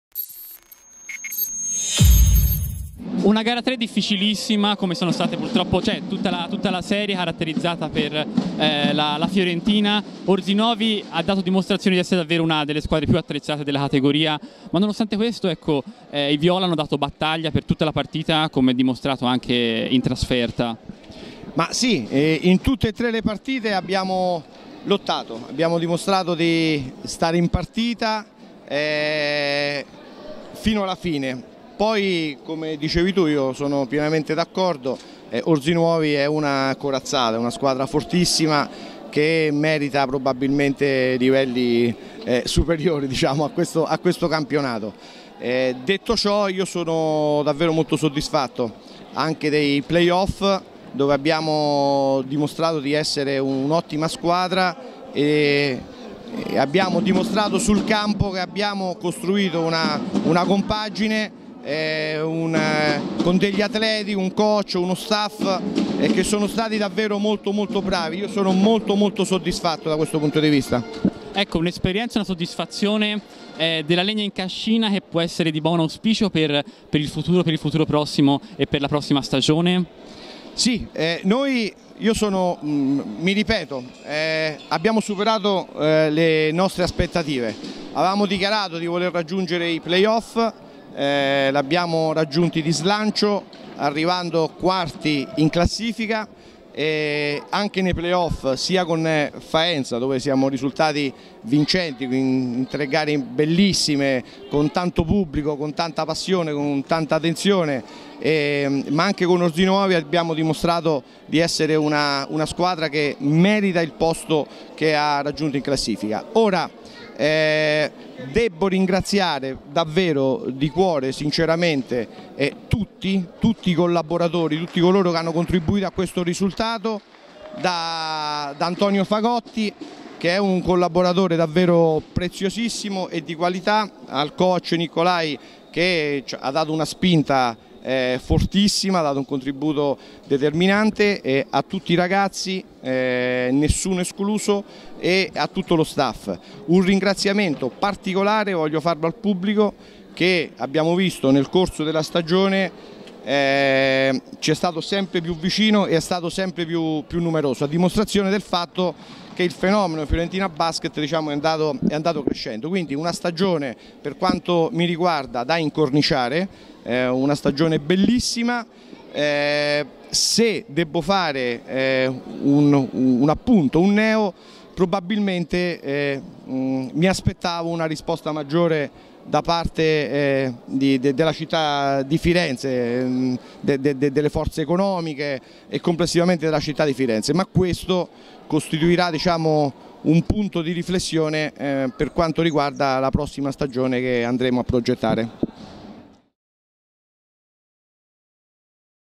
Il presidente della Fiorentina Piacenti dopo la chiusura della stagione
Intervista di Francesco Guidotti